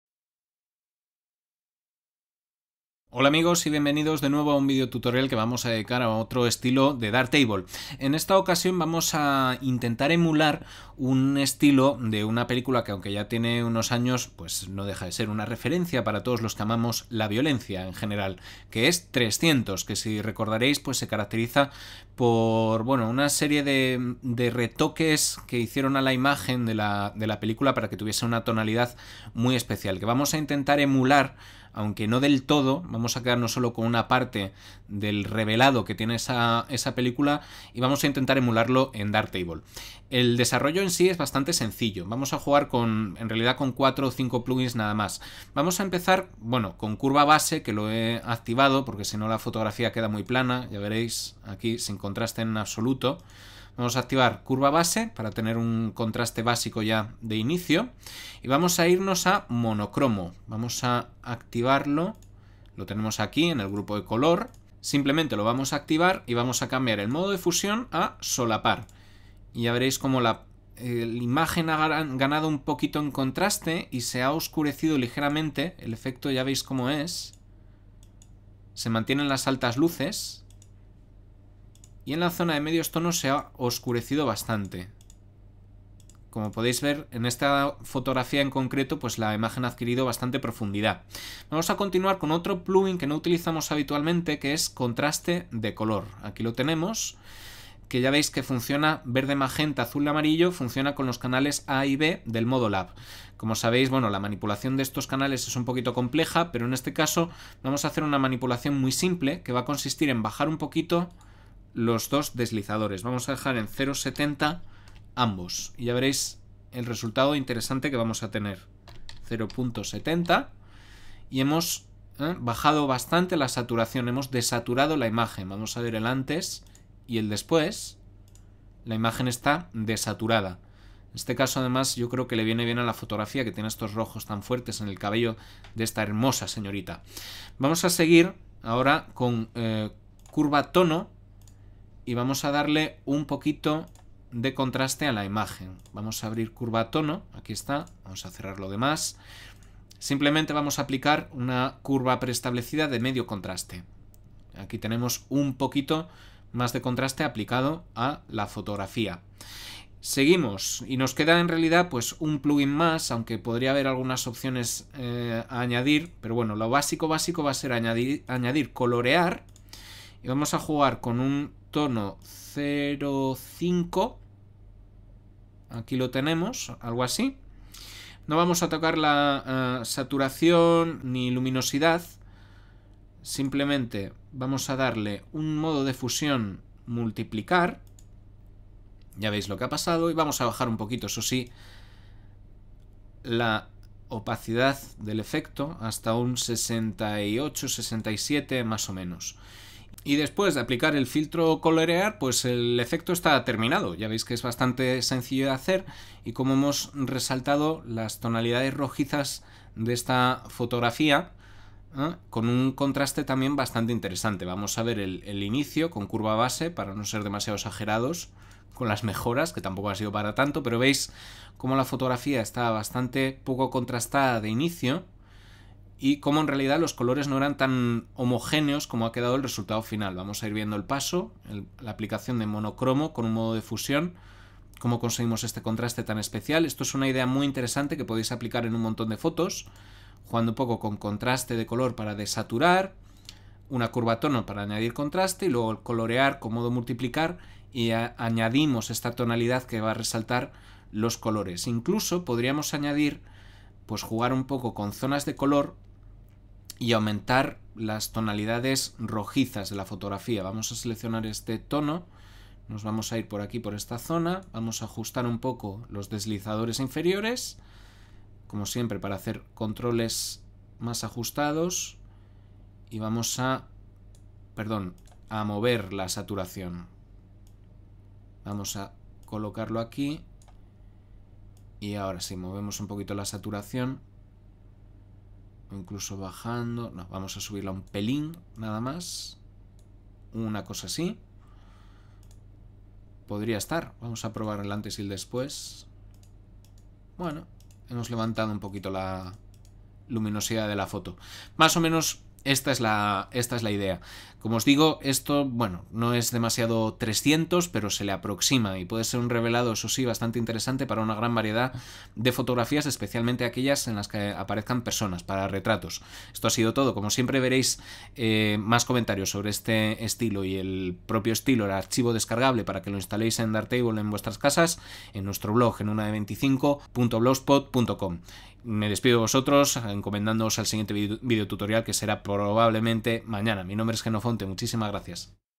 Estilos Darktable: 300
Todavía no he llegado a los 300 estilos, ni a los 300 suscriptores... esto es un estilo basado en la película homónima, no os liéis.